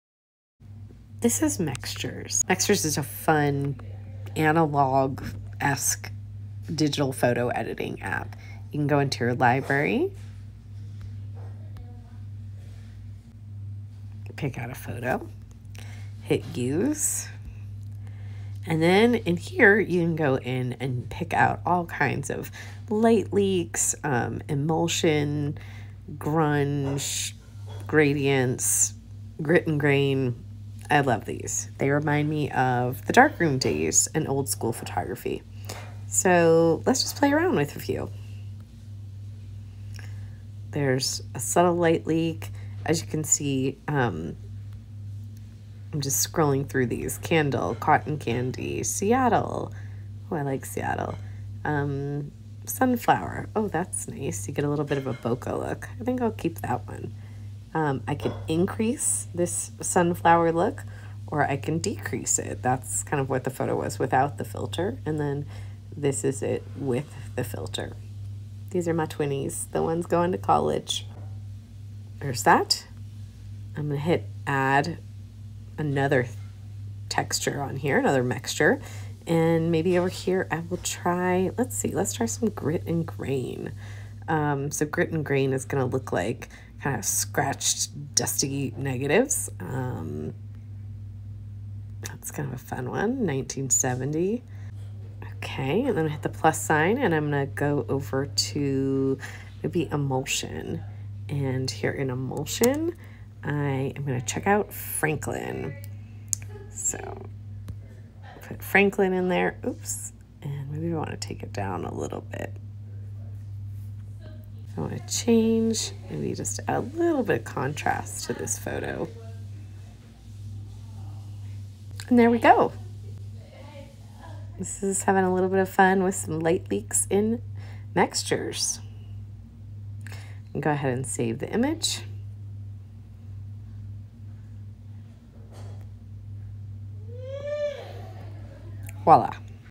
How I edit iPhone photos to look like film in under 5 minutes
How to use Mextures to edit iPhone photos into cool, vintage, retro film inspired photos.